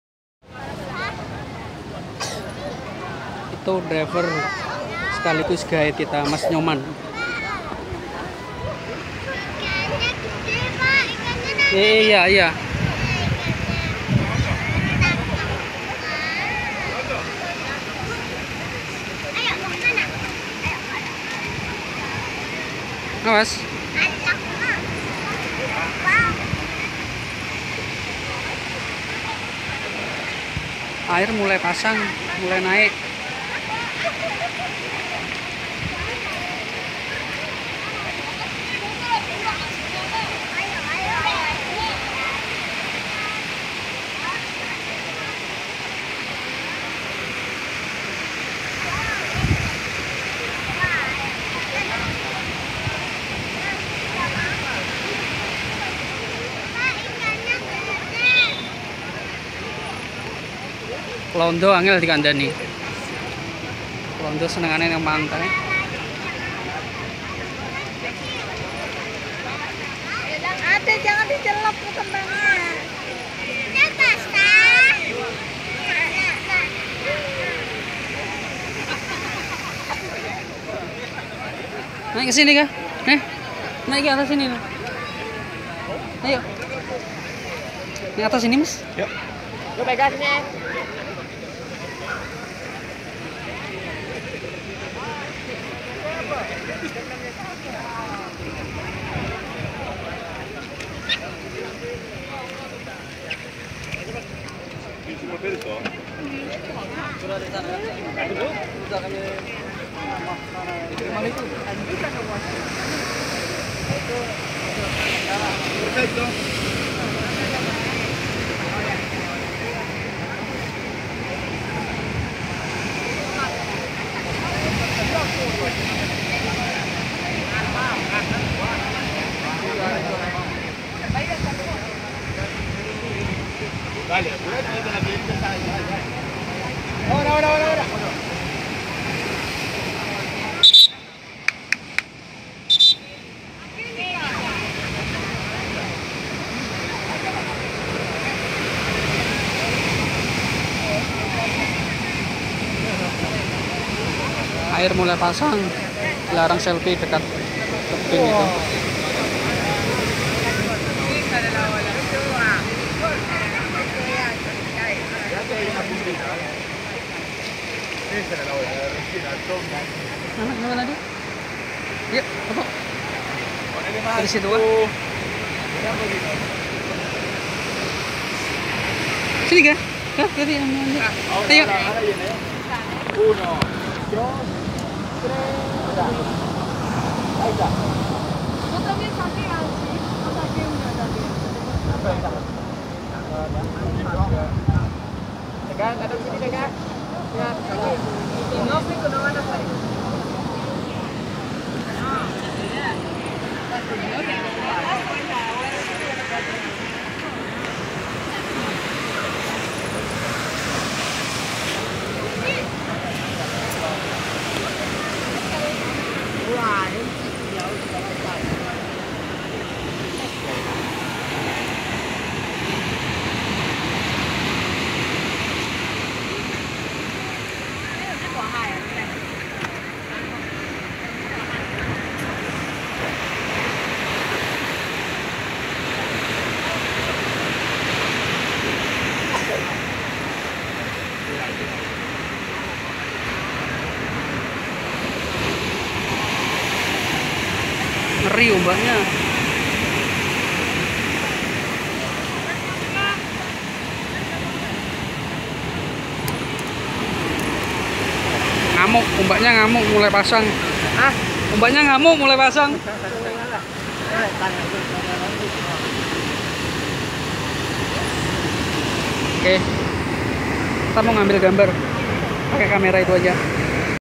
Pura Tanah Lot Tabanan Bali news 2017
Ombak yg mengerikan namun mengasyikan cukup indah & cantik. Air mulai pasang